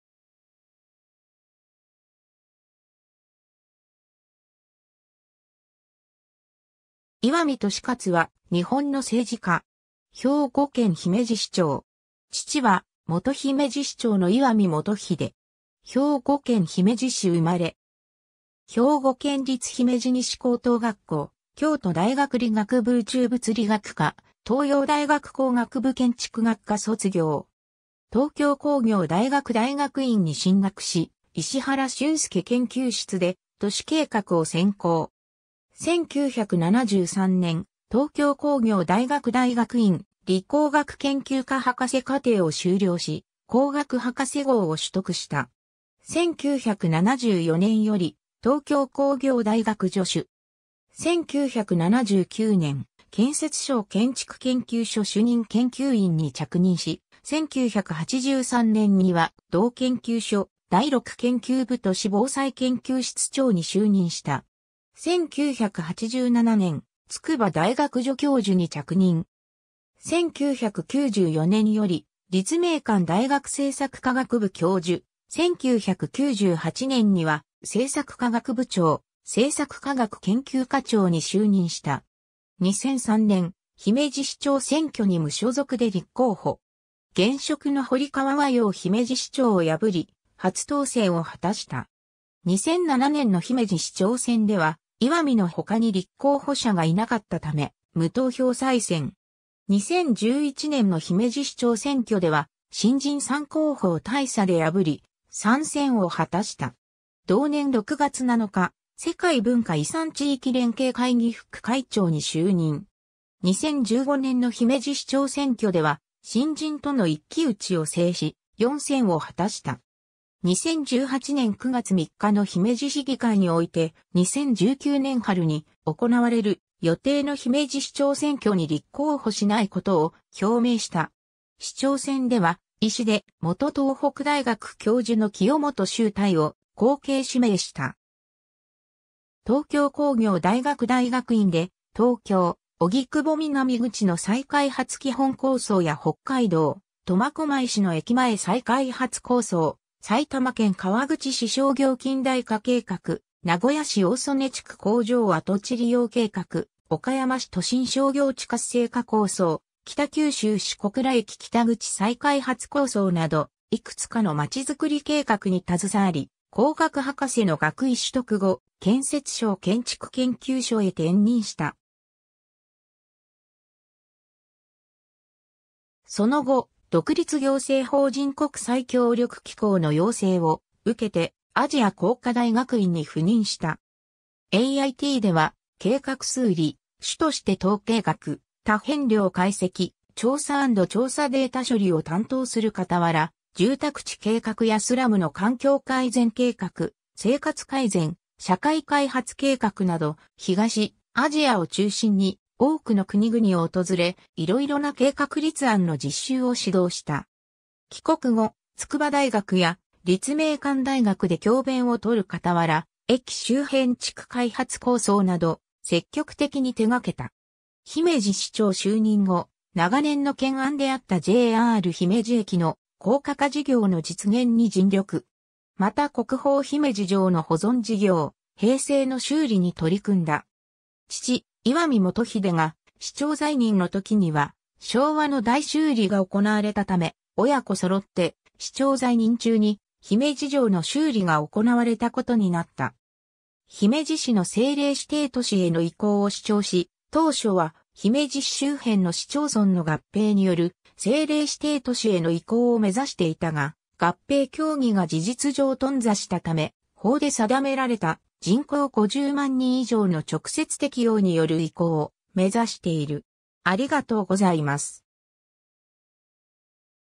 石見利勝
石見 利勝（いわみ としかつ、1941年8月14日 - ）は、日本の政治家。
兵庫県姫路市長（4期）。
 父は元姫路市長の石見元秀。
 兵庫県姫路市生まれ。
兵庫県立姫路西高等学校、京都大学理学部宇宙物理学科、東洋大学工学部建築学科卒業。
東京工業大学大学院に進学し、石原舜介研究室で都市計画を専攻。
1973年、東京工業大学大学院理工学研究科博士課程を修了し、工学博士号を取得した。
1974年より東京工業大学助手。
1979年、建設省建築研究所主任研究員に着任し、1983年には同研究所第六研究部都市防災研究室長に就任した。
1987年、筑波大学助教授に着任。
1994年より立命館大学政策科学部教授、1998年には政策科学部長、政策科学研究科長に就任した。
 2003年、姫路市長選挙に無所属で立候補。
現職の堀川和洋姫路市長を破り、初当選を果たした。
2007年の姫路市長選では、石見の他に立候補者がいなかったため、無投票再選。
2011年の姫路市長選挙では、新人3候補を大差で破り、3選を果たした。
同年6月7日、世界文化遺産地域連携会議副会長に就任。
2015年の姫路市長選挙では、新人との一騎討ちを制し、4選を果たした。
 2018年9月3日の姫路市議会において、2019年春に行われる予定の姫路市長選挙に立候補しないことを表明した。
市長選では、医師で元東北大学教授の清元秀泰を後継指名した。
 東京工業大学大学院で、東京・荻窪南口の再開発基本構想や北海道苫小牧市の駅前再開発構想、埼玉県川口市商業近代化計画、名古屋市大曽根地区工場跡地利用計画、岡山市都心商業地活性化構想、北九州市小倉駅北口再開発構想など、いくつかのまちづくり計画に携わり、工学博士の学位取得後、建設省建築研究所（現：独立行政法人建築研究所）へ転任した。
 その後、独立行政法人国際協力機構 (JICA) の要請を受けてアジア工科大学院 (AIT) に赴任した。
AITでは、計画数理、主として統計学、多変量解析、調査＆調査データ処理を担当する傍ら、住宅地計画やスラムの環境改善計画、生活改善・社会開発計画など、東アジアを中心に多くの国々を訪れ、色々な計画立案の実習を指導した。
帰国後、筑波大学や立命館大学で教鞭をとる傍ら、駅周辺地区開発構想など積極的に手掛けた。
 姫路市長就任後、長年の懸案であったJR姫路駅の高架化事業の実現に尽力。
また国宝姫路城の保存事業「平成の修理」に取り組んだ。
父・石見元秀が市長在任の時には「昭和の大修理」が行われたため親子揃って市長在任中に姫路城の修理が行われたことになった。
姫路市の政令指定都市への移行を主張し、当初は姫路市周辺の市町村の合併による政令指定都市への移行を目指していたが、合併協議が事実上頓挫したため、法で定められた「人口50万人以上」の直接適用による移行を目指している。